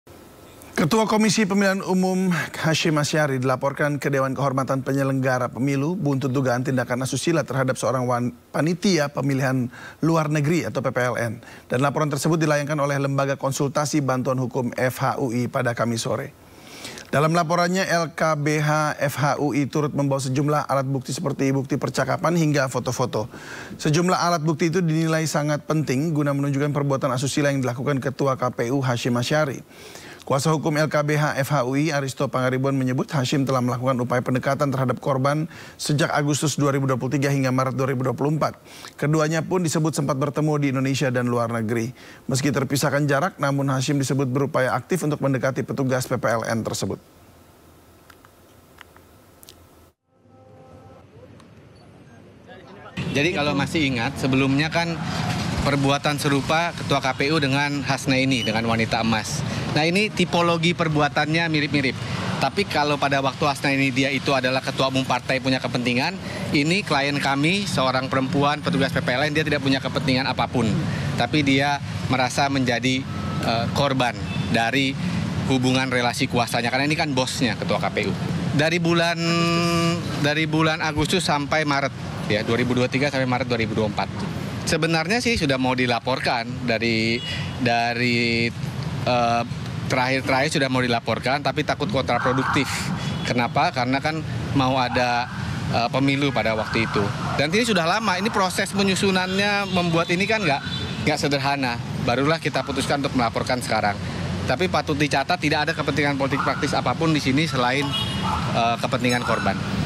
Ketua KPU RI Kembali Diadukan ke DKPP Terkait Dugaan Asusila - Sindo Prime 18/04
Ketua Komisi Pemilihan Umum (KPU) RI Hasyim Asy’ari diadukan ke Dewan Kehormatan Penyelenggara Pemilu (DKPP) RI atas dugaan tindakan asusila terhadap panitia penyelenggara luar negeri (PPLN).